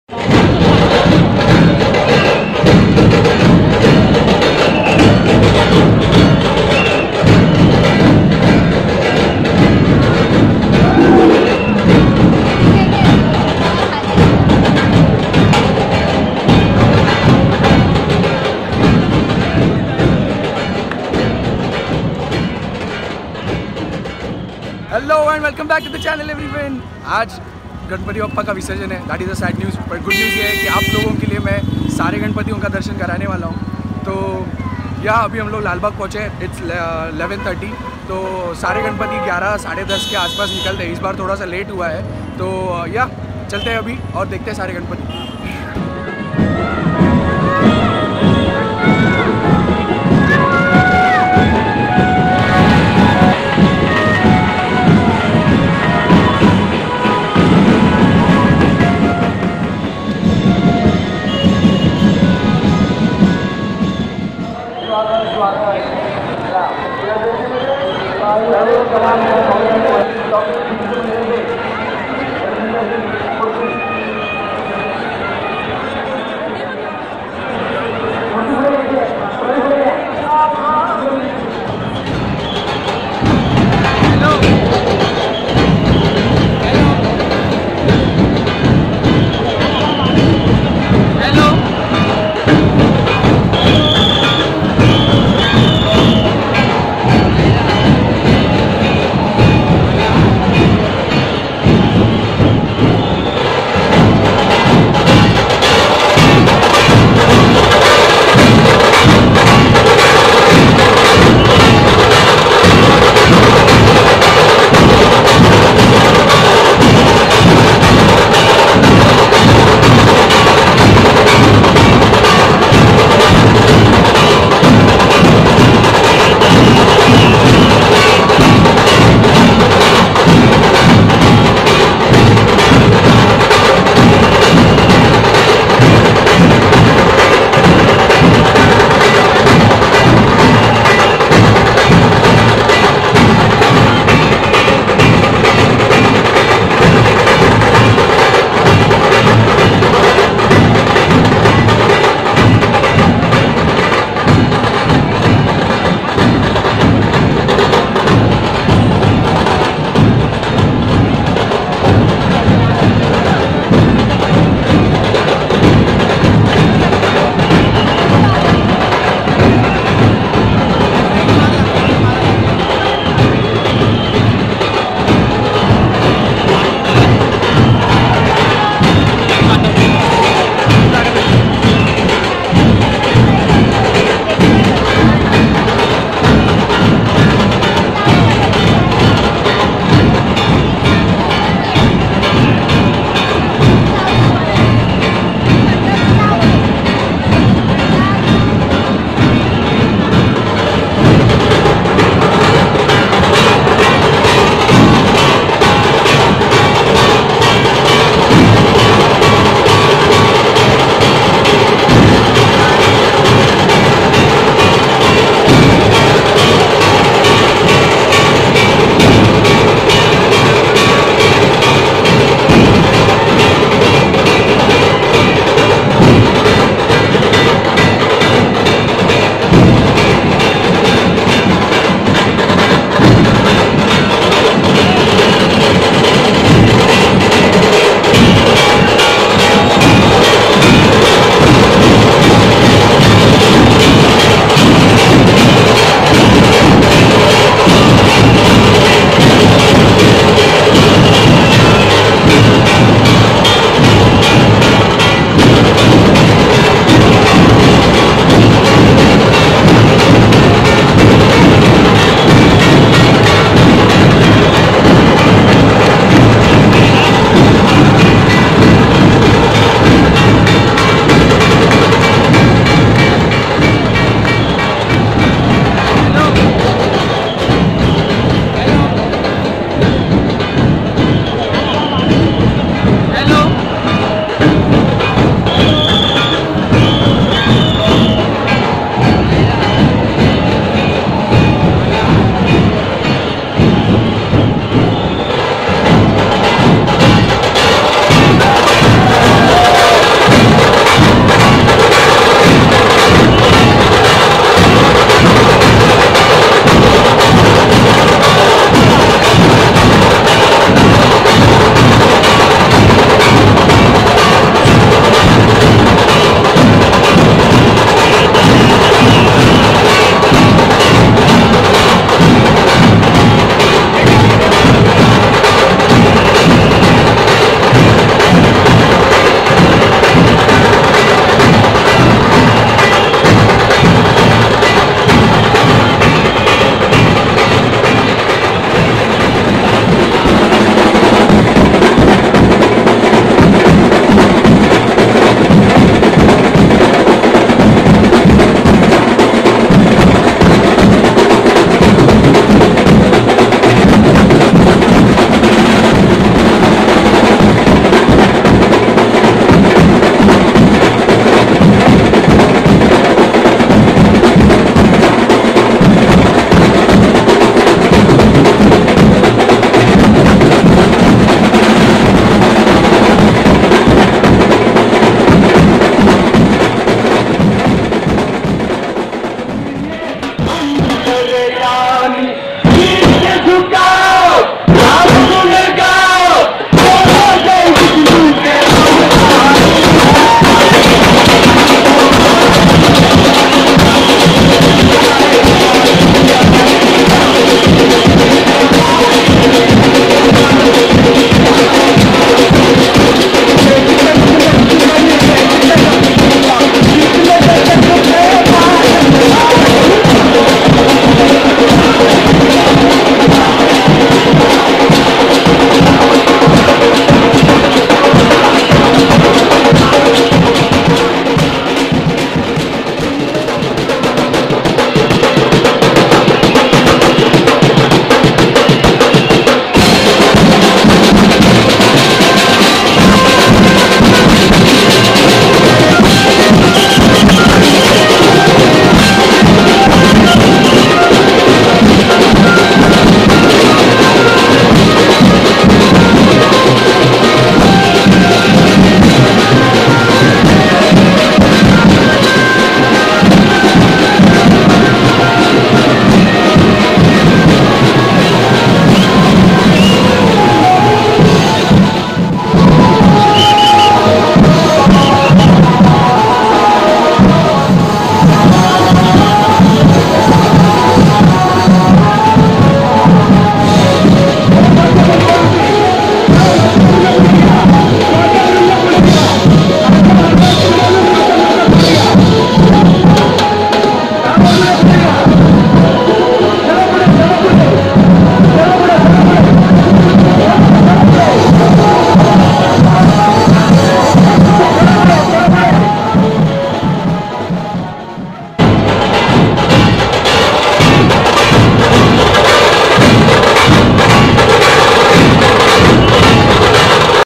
Mumbai Ganpati visarjan 2018
It is a grand celebration in Mumbai on last day of Ganpati, Its fun to be there and enjoy each and every moment

If you want me to cover anything do let me know in comment section, I will cover that in my vlogs.